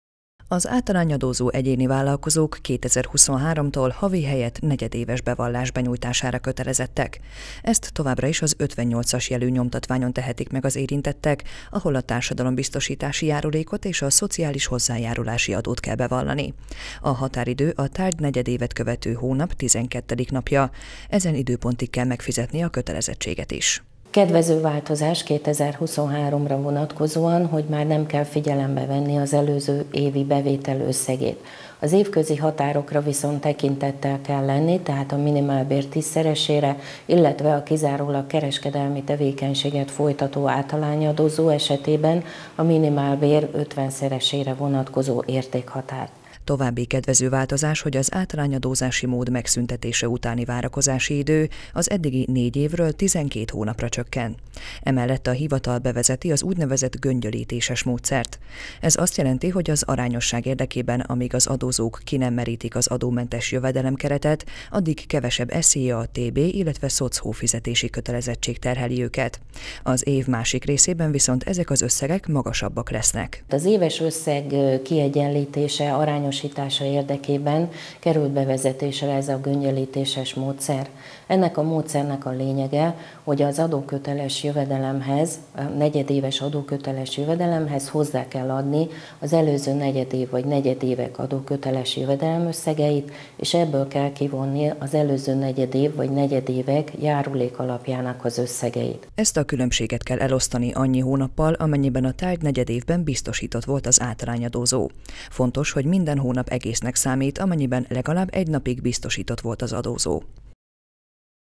Kedvező változás az átalányadózásban
Kedvezően változnak az átalányadózás szabályai 2023. január 1-jétől. Az érintetteknek továbbra is be kell nyújtaniuk a járulékbevallásukról készült dokumentumot, azonban már nem havonta, hanem negyedévente. Ugyanígy a fizetési kötelezettséget is ekkor kell teljesíteni. Újítás még, hogy az arányosítás érdekében az adómentes jövedelemhatárig kevesebb adót, illetve járulékot kell fizetniük az átalányadózóknak.